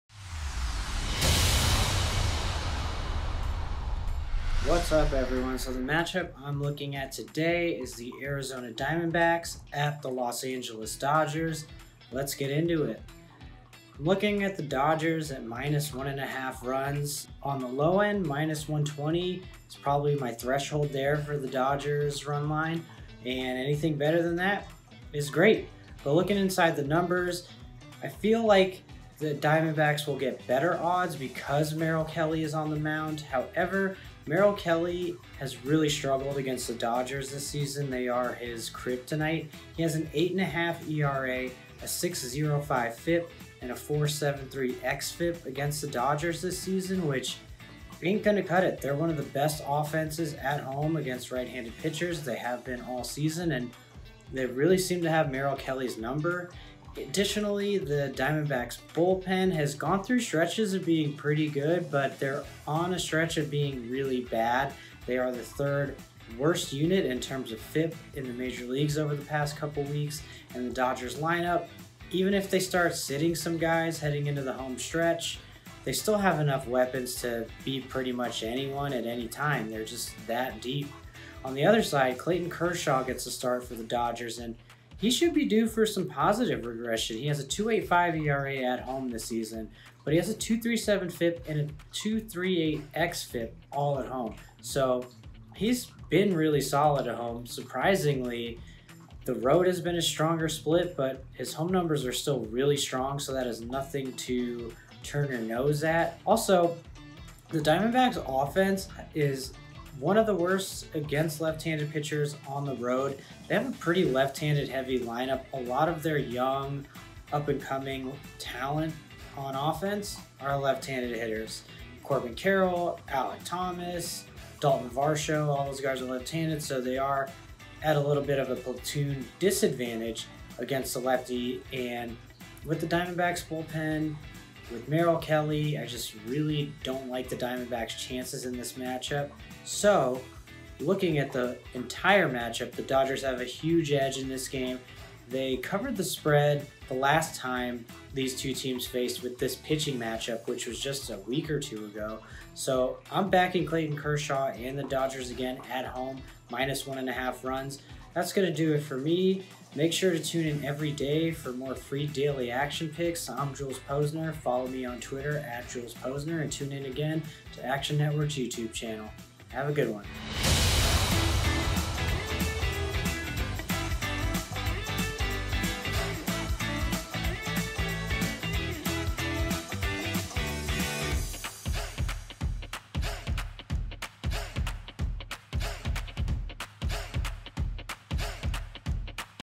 Arizona Diamondbacks vs Los Angeles Dodgers Picks & Predictions | MLB Best Bets for Monday, 9/19/22
MLB best bets from Action Network expert Jules Posner. Jules breaks down the Arizona Diamondbacks vs Los Angeles Dodgers game in a rematch between Merrill Kelly and Clayton Kershaw. Free MLB picks for Monday, 9/19/22!